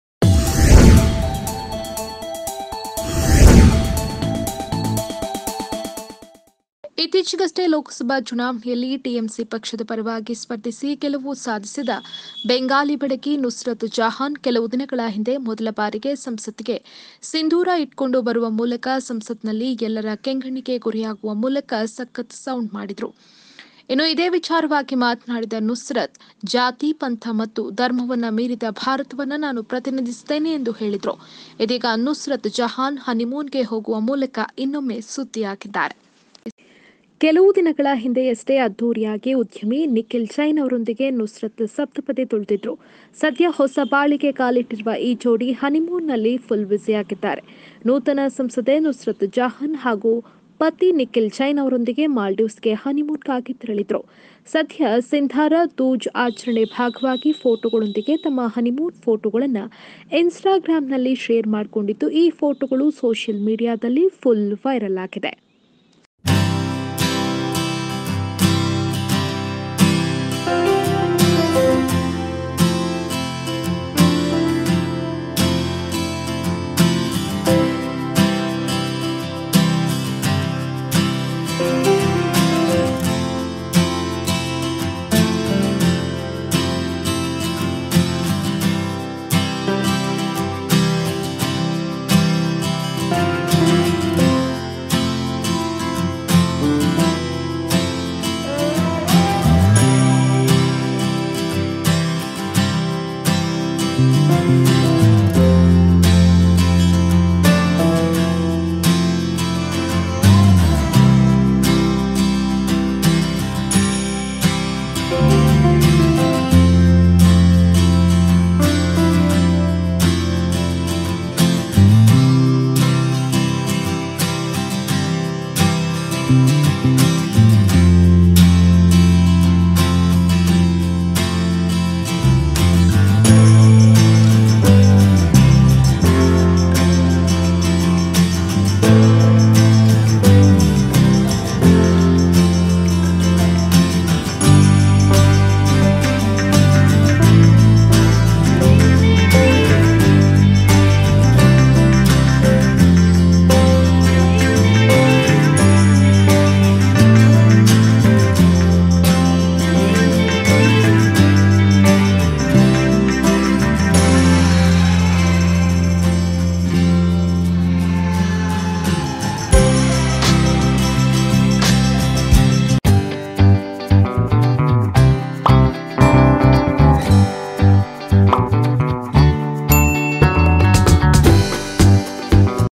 Nusrat Jahan's dazzling honeymoon pictures from Mauritius are unmissable! | TVNXT Kannada
Watch Nusrat Jahan's dazzling honeymoon pictures from Mauritius are unmissable! | Nusrat | TVNXT Kannada